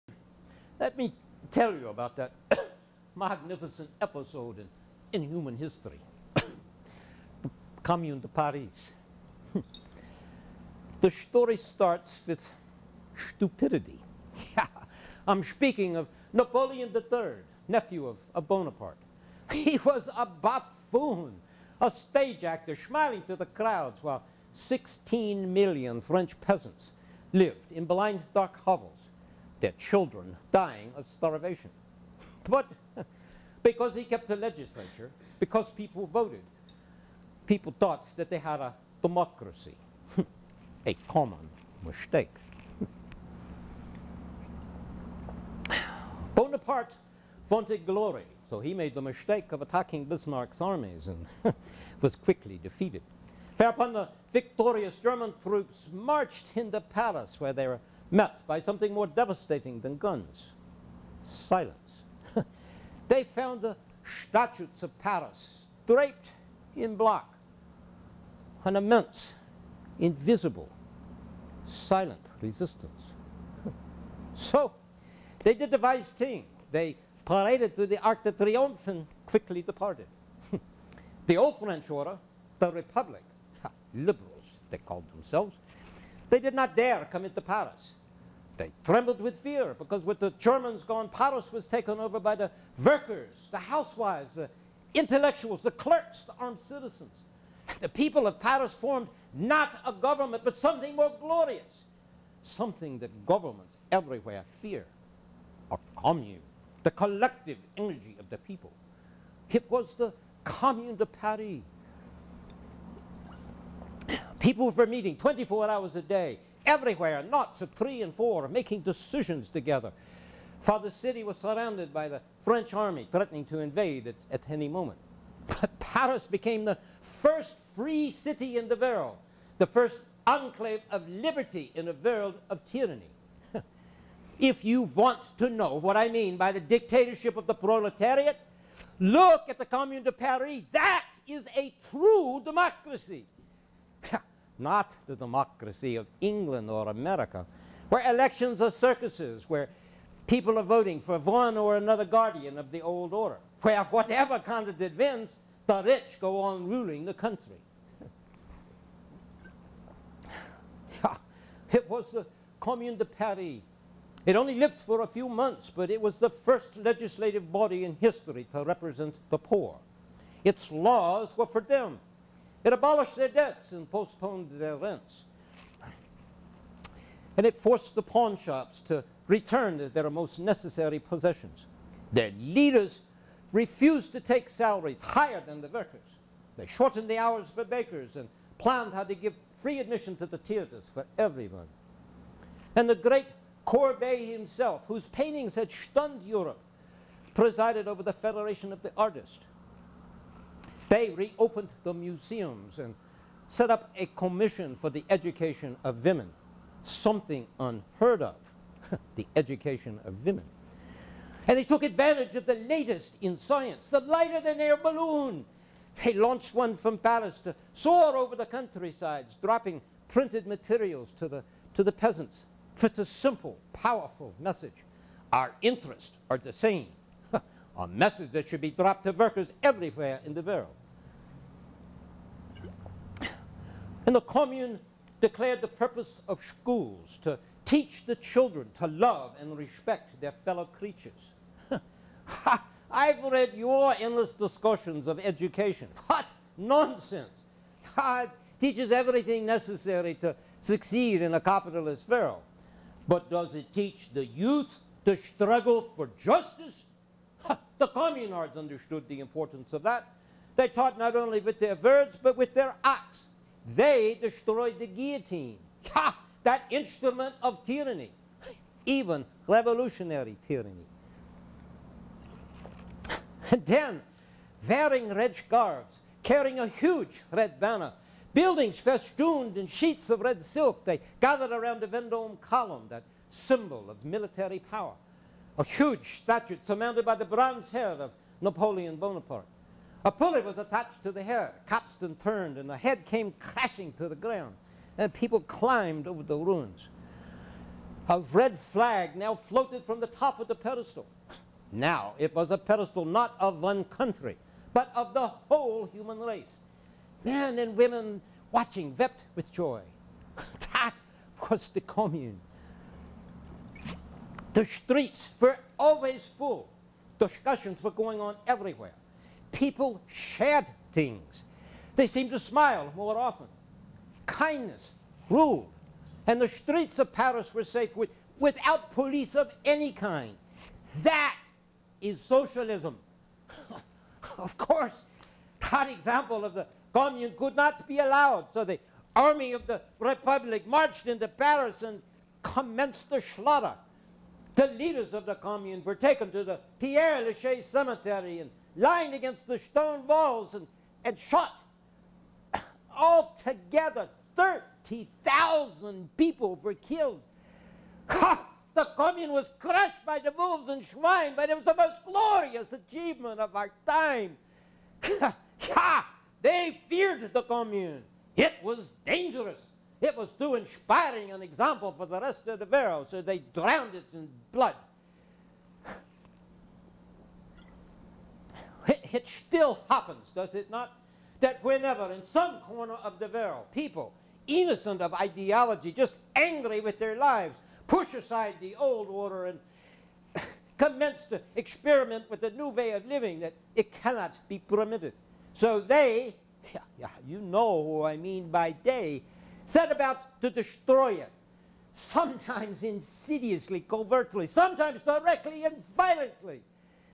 Marx on Commune de Paris
Karl Marx tells the story of the Commune de Paris, the greatest achievement of working class people, a true democracy. From play Marx in Soho.